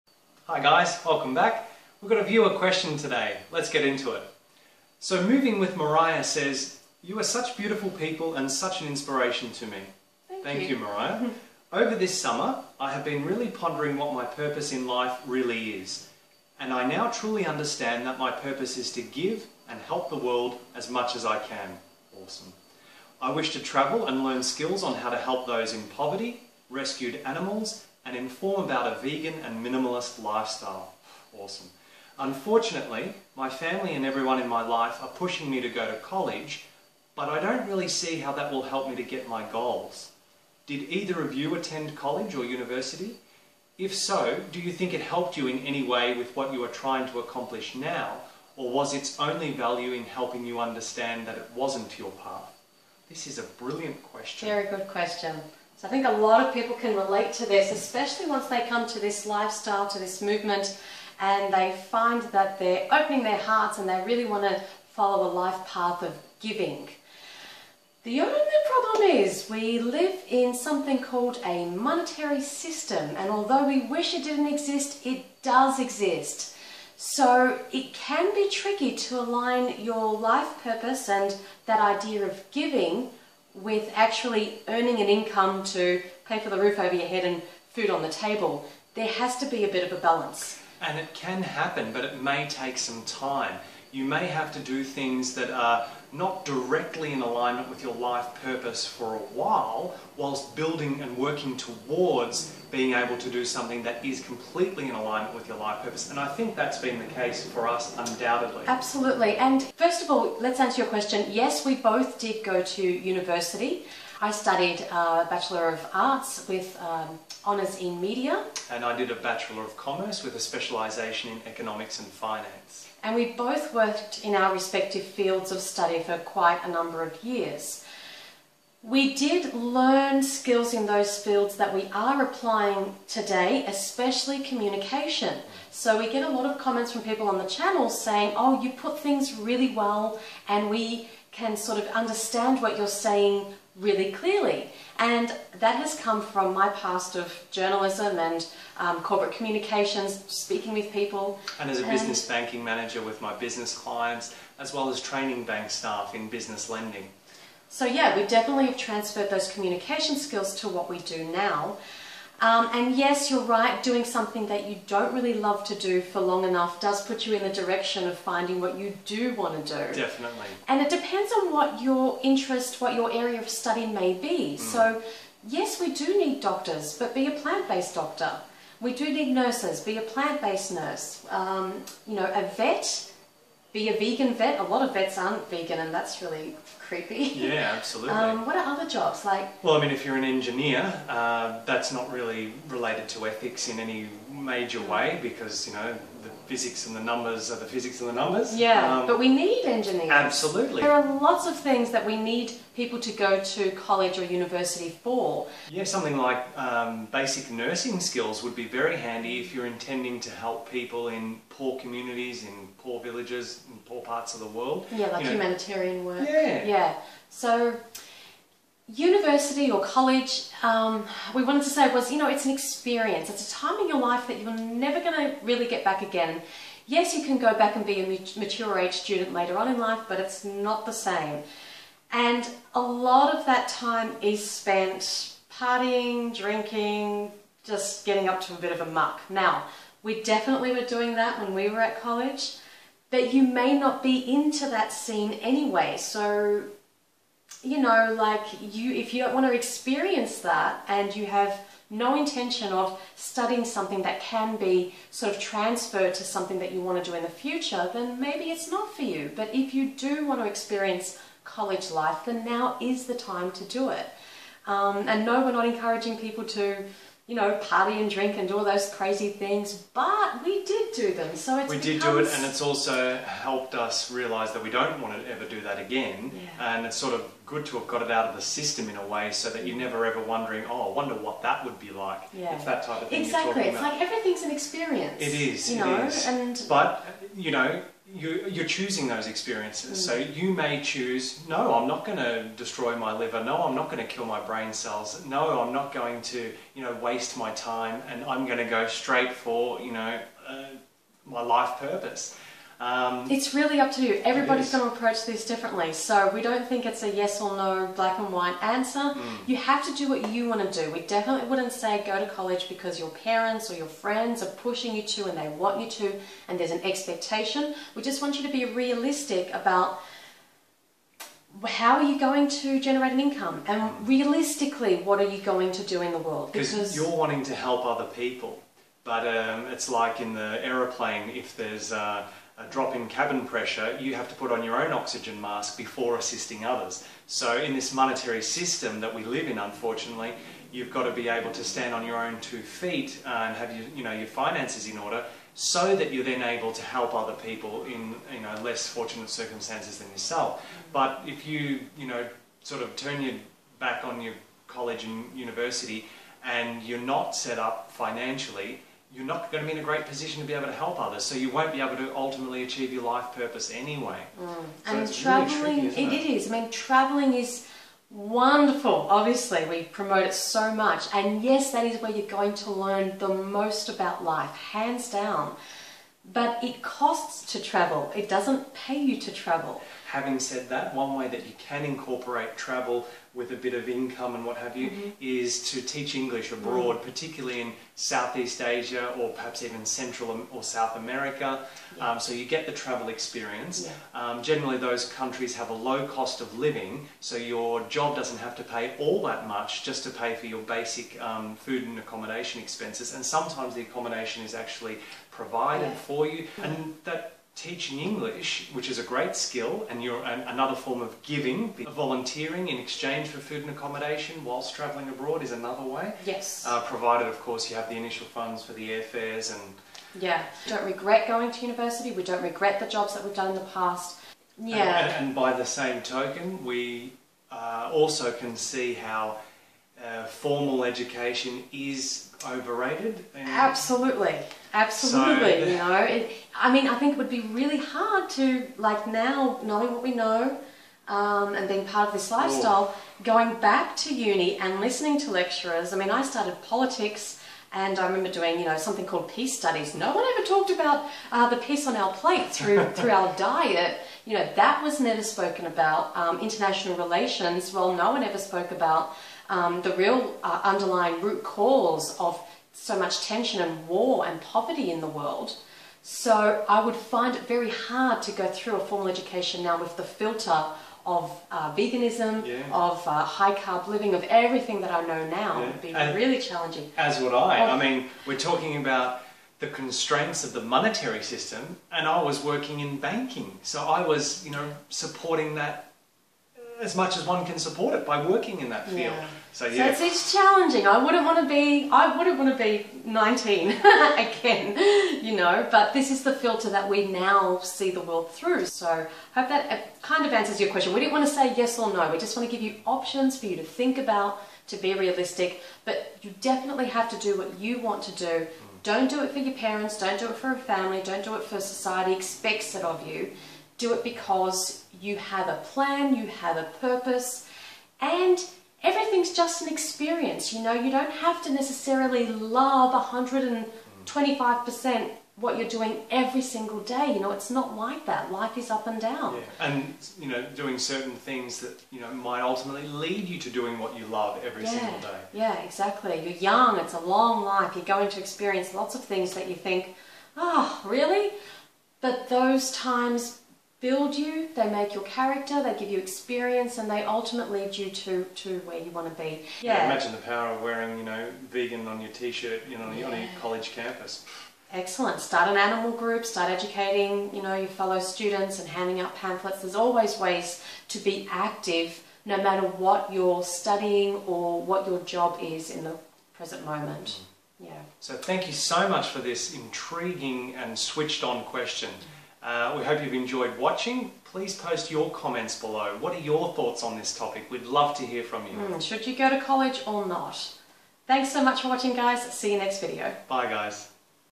Should I go to college?
Viewer question: My family is pushing me to go to college but I don’t see how it will help me get to my goals. Should I attend college?

If you enjoy this video please press the ‘LIKE’ button and ‘SHARE’ it with your social media networks.